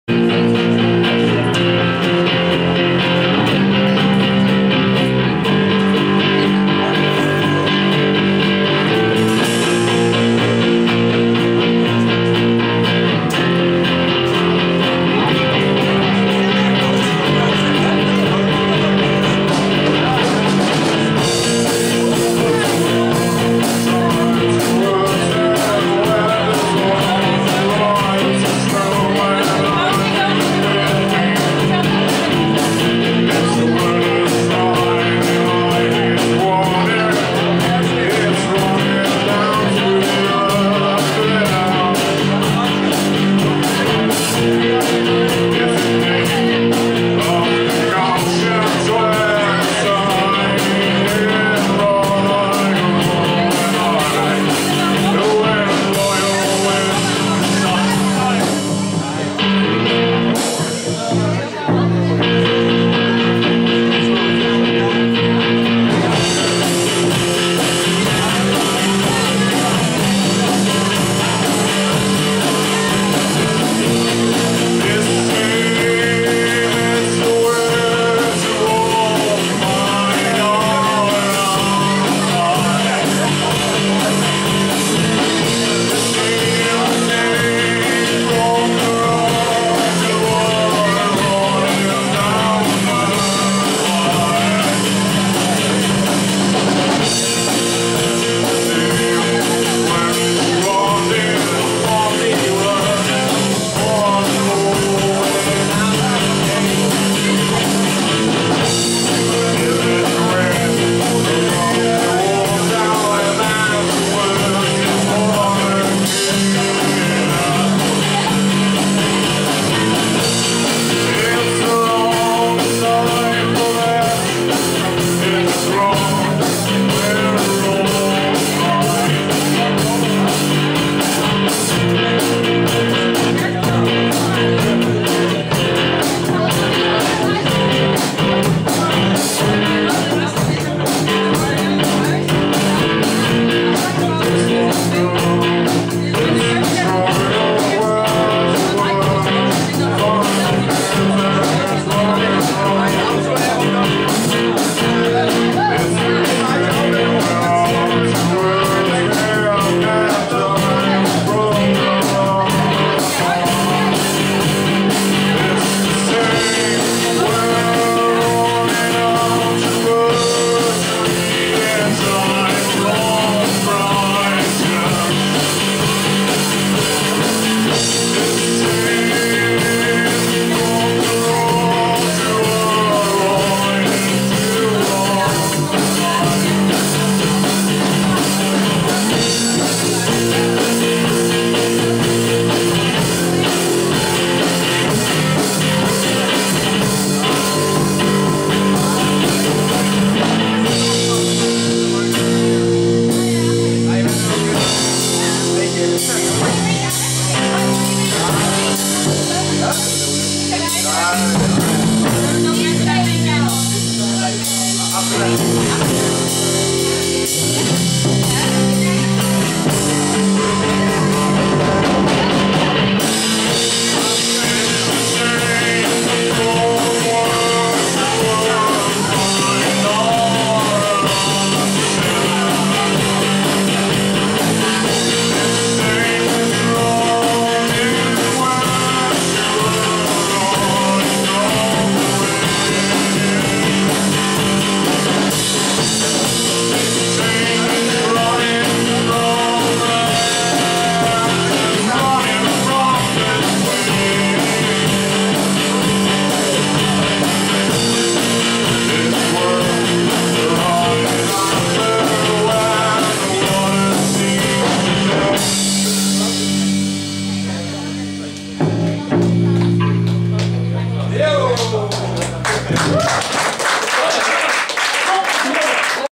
JamScape Sessie Escape Veenendaal 19-10-2013 Own Song Jam 2
De JamScape Sessies worden georganiseerd door Jongerencentrum Escape Veenendaal en de band The Axes.

THE JAMSCAPE SESSIONS vinden maandelijks plaats op zaterdagavond in Pop en Cultuurpodium Escape. Talentontwikkeling in een ontspannen en creatieve sfeer staat centraal. Tijdens de sessies is de bar geopend voor muziek minnende bezoekers.